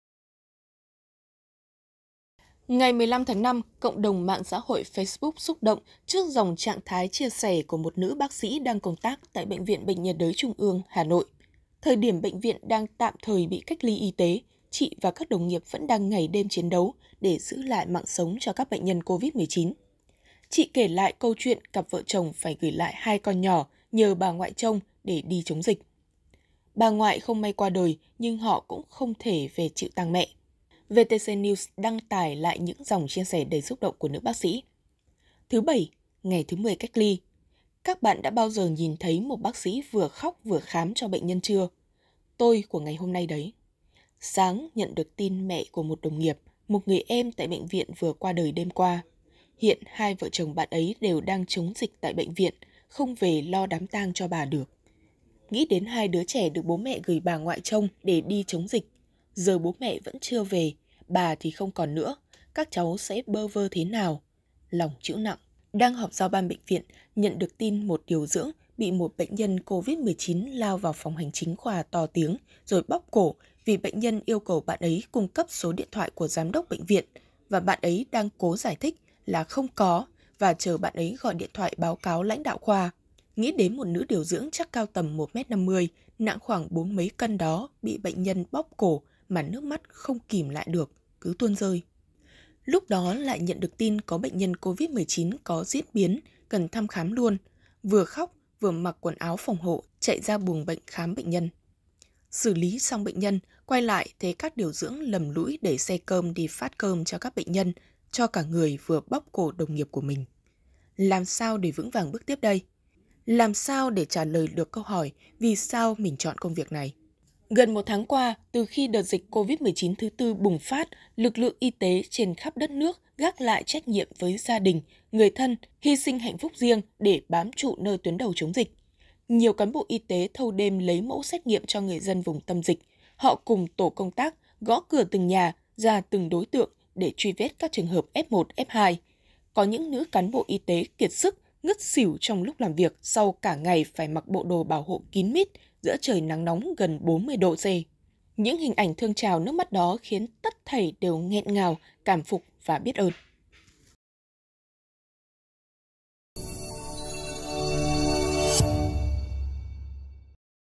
Nhói lòng bác sĩ tham gia tuyến đầu chống dịch, mẹ mất không thể về chịu tang
Đang tham gia chống dịch tại BV Bệnh Nhiệt đới TW thì hai vợ chồng bác sĩ nhận được tin mẹ mất nhưng không thể về chịu tang.